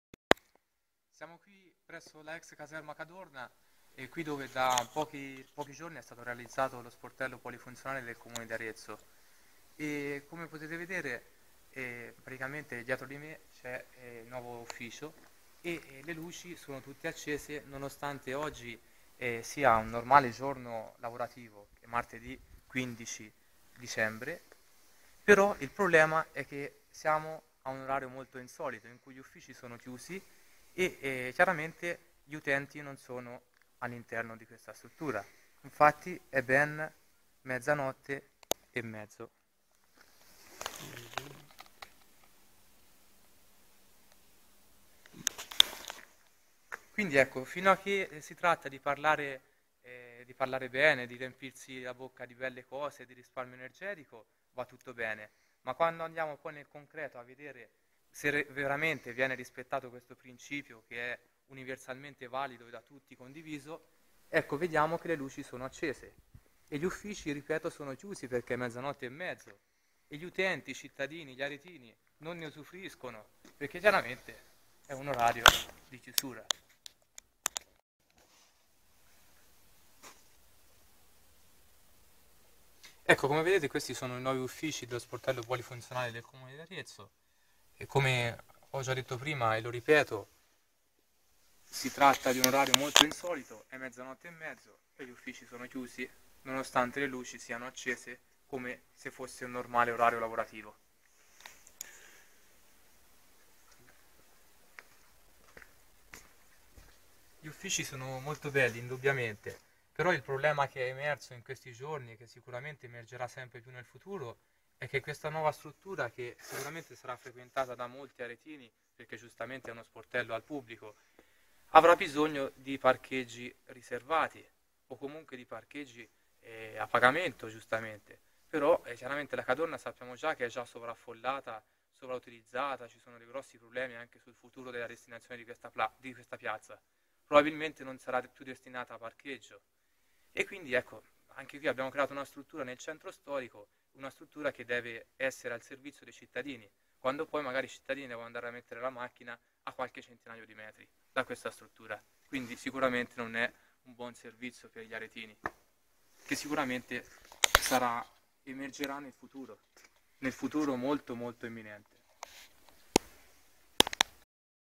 Arezzo Ex caserma Cadorna...predicar bene e razzolar male!
Si parla tanto di risparmio energetico e poi. si veda gli uffici dello sportello unico del comune ad Arezzo nell'ex piazzale Cadorna